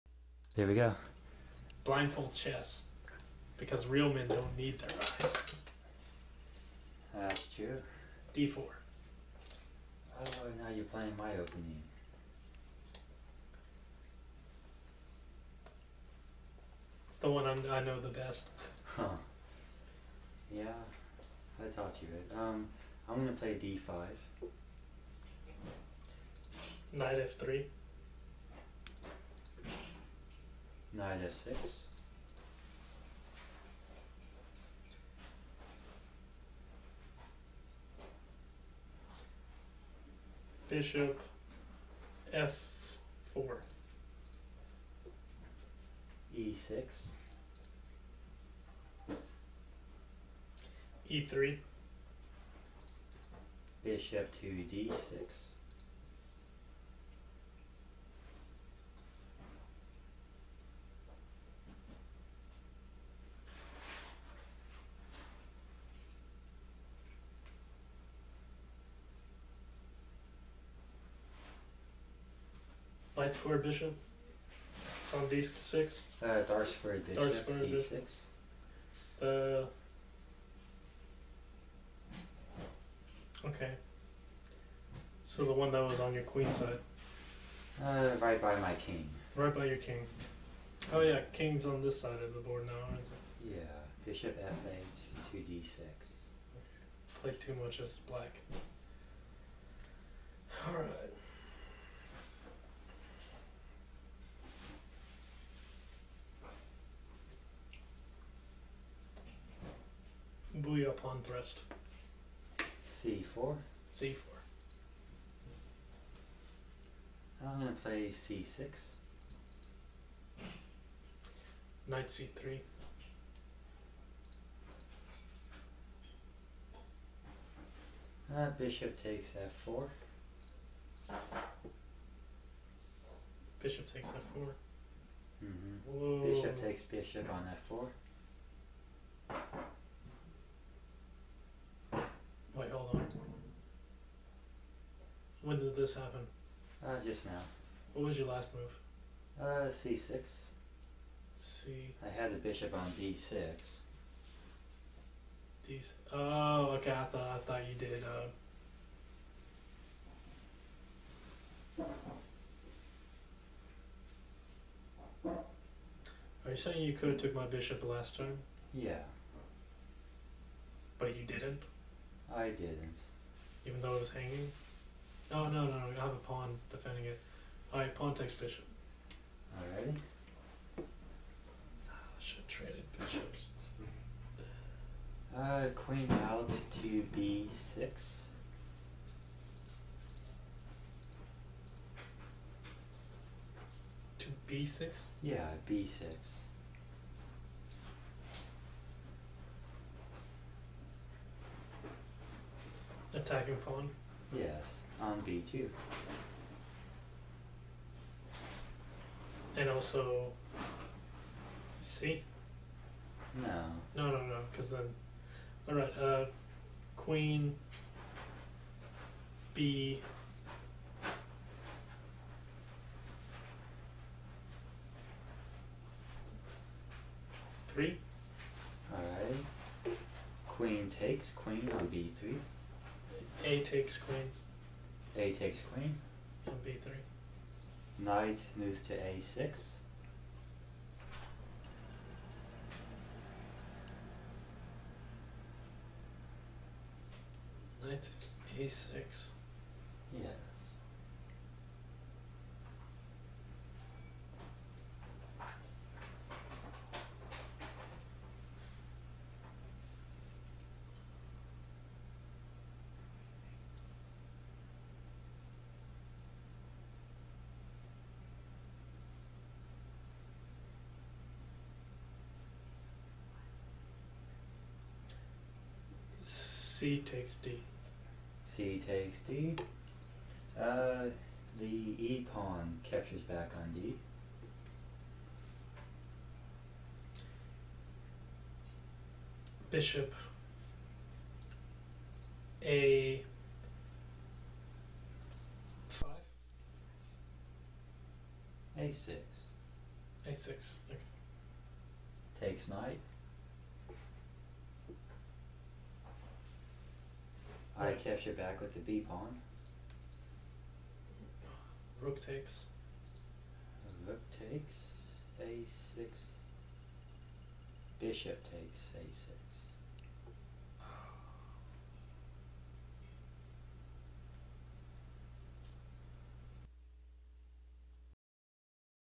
Blindfold chess: Terminator Bhuiyan vs Expert David Luscomb
We switched places and my friend played blindfolded against me the expert. He is getting the hang of blindfold chess but did blunder in the opening dropping a piece.